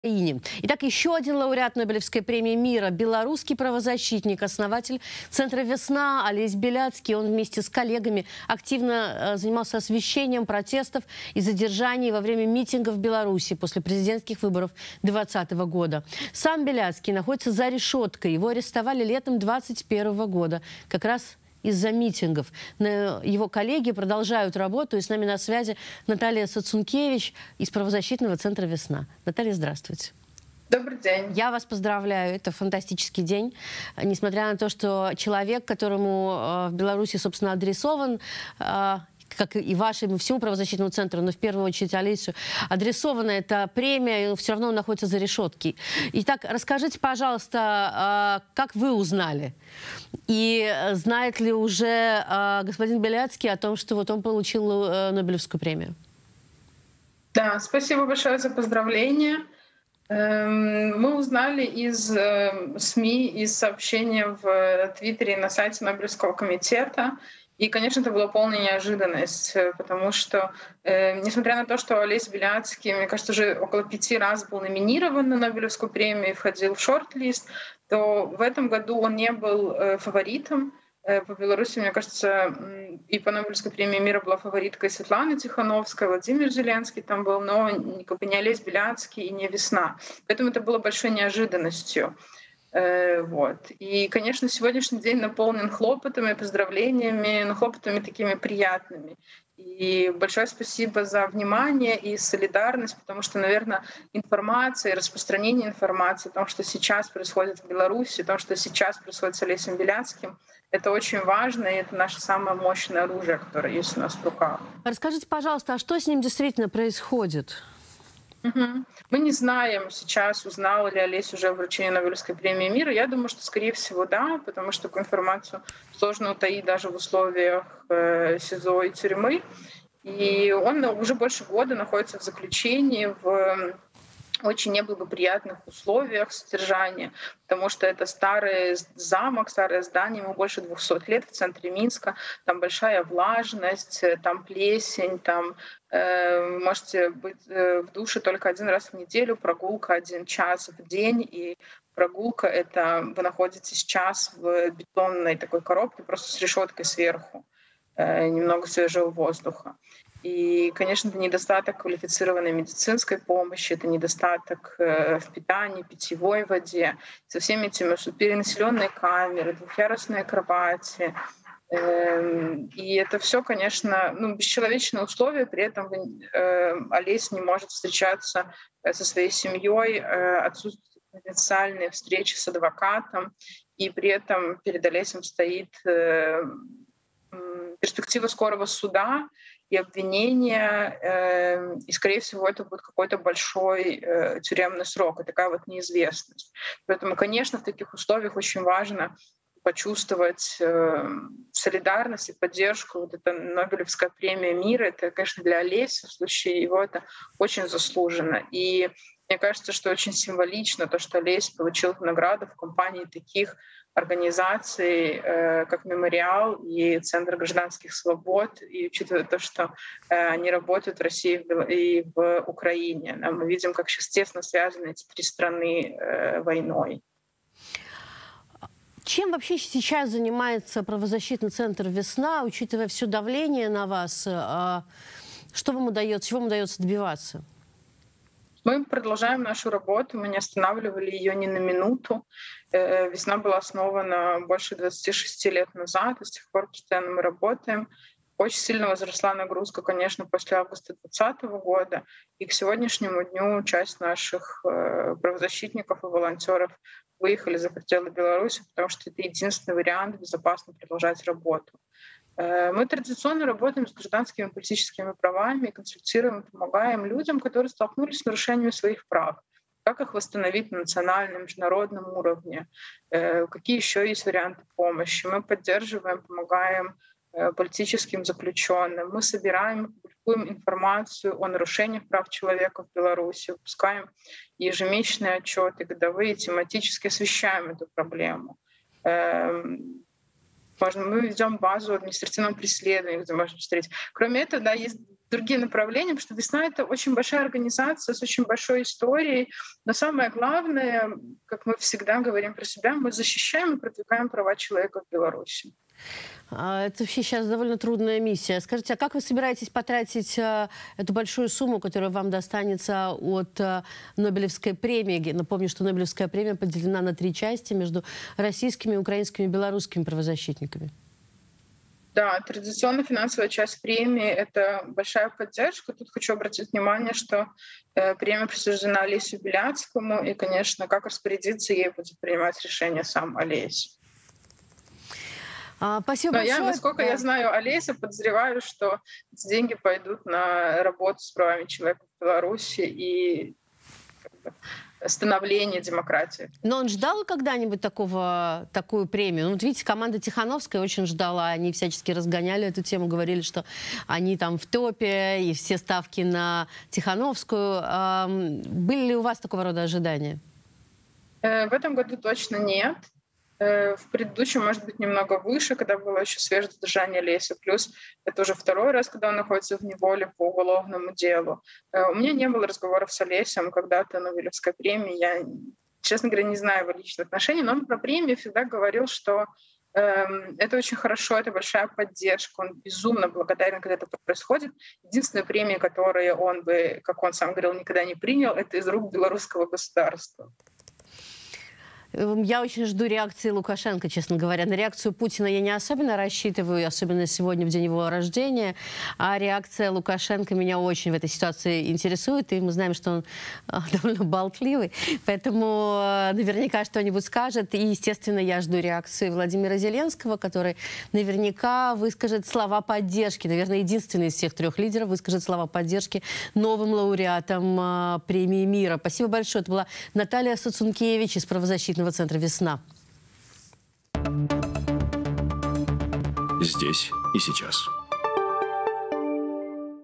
Нобелевская премия мира правозащитникам: как в белорусском движении «Весна» отреагировали на награду
Белорусский правозащитник Алесь Беляцкий — лауреат Нобелевской премии мира. 7 октября Нобелевский комитет вручил премию ему, а также российскому «Мемориалу» и украинскому «Центру гражданских свобод». О том, как отреагировали на Нобелевскую премию в основанном Беляцким движении «Весна» в эфире Дождя рассказала одна из его сотрудниц.

Следите за главными новостями и новыми выпусками программ в телеграм-канале Дождя